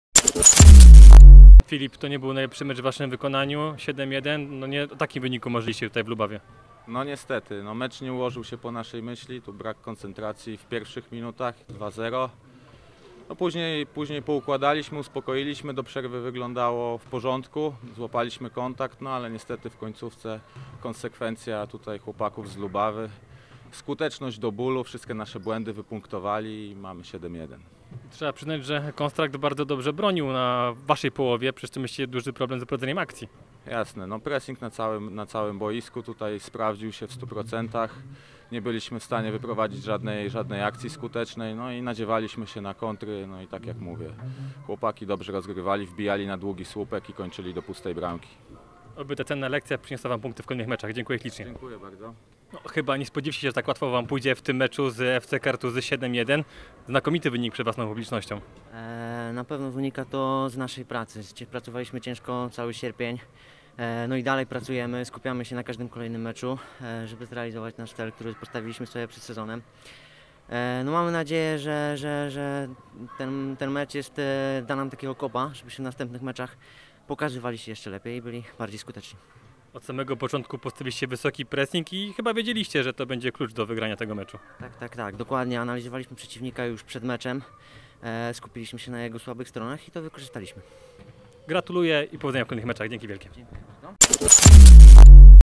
Constract Lubawa - FC Kartuzy / Wypowiedzi / 1 Polska Liga Futsalu [13.10.2018]
Zobacz wypowiedzi po meczu 1 Polskiej Ligi Futsalu: Constract Lubawa - FC Kartuzy z KP Sport